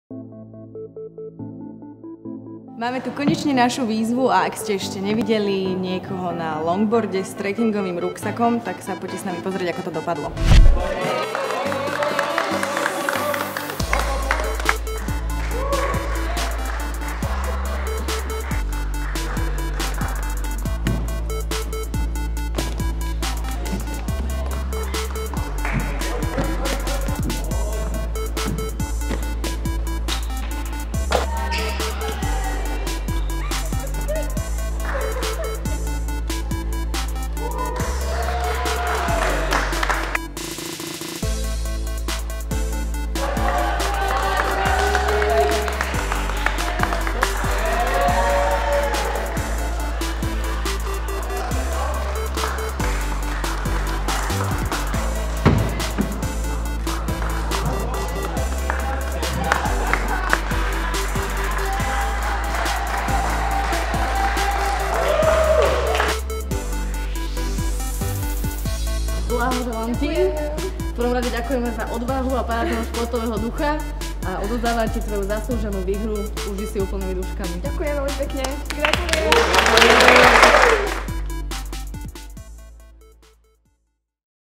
Intersport Výzva - Duel s Bašou Števulovou
Sleduj posledný duel z Intersport Výzvy. Slalom na longboarde nie je žiadna malina, obzvlášť, keď to máš sťažené prekážkovým behom.